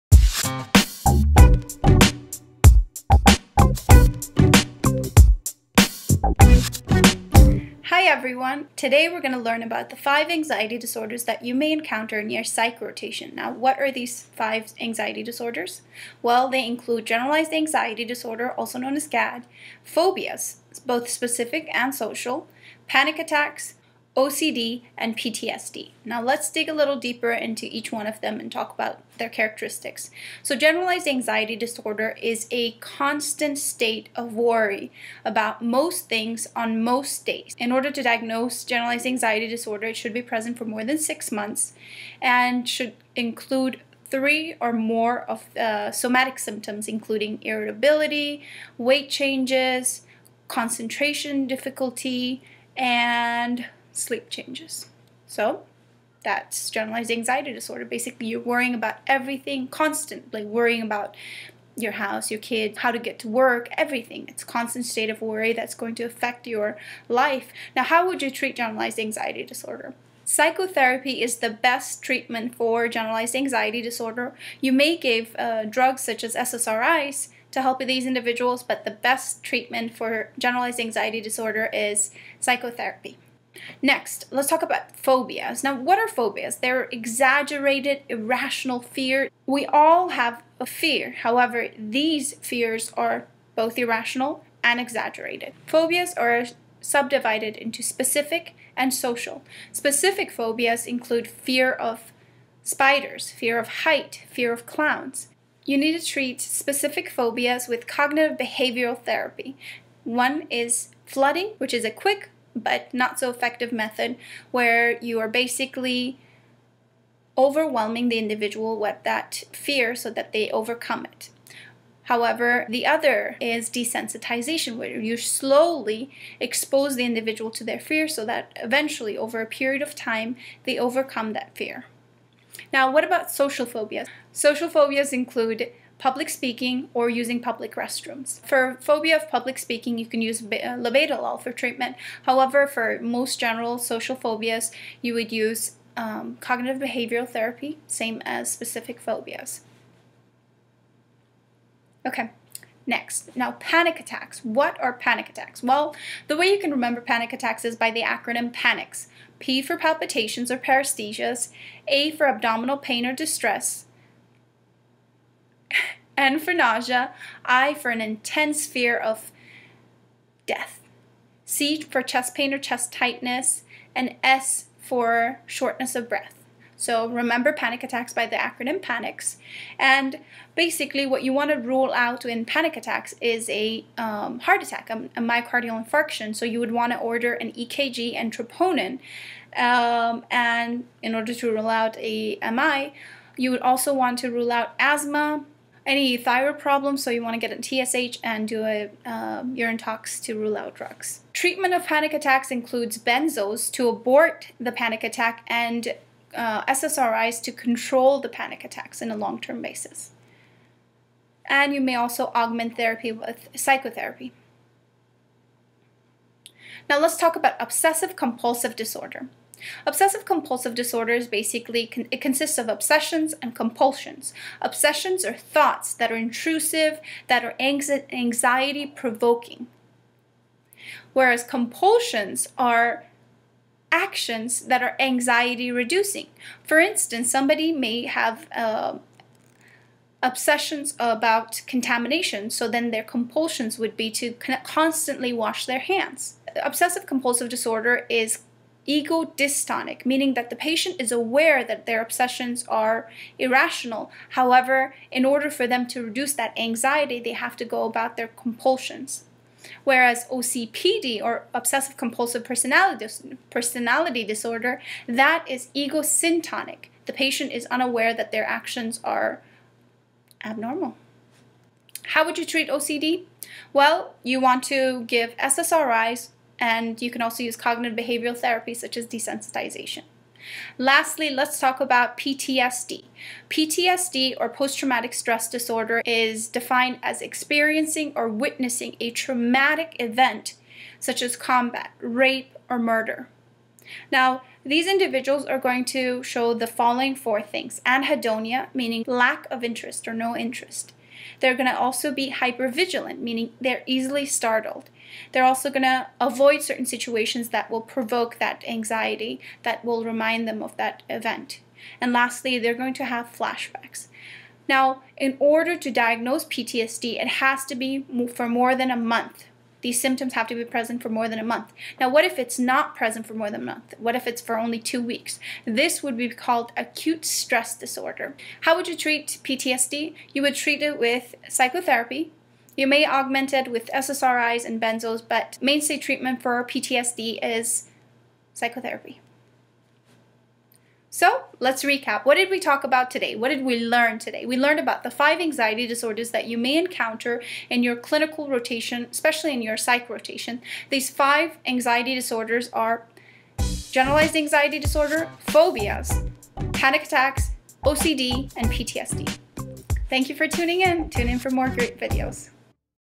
Anxiety Disorders explained in 5 mins!!!
There are 5 anxiety disorder that you may encounter during rotations especially during Psych. These include: GAD, Phobias, Panic Attacks, OCD, & PTSD. This short videos explains each one, how to diagnose these disorders and how to treat them.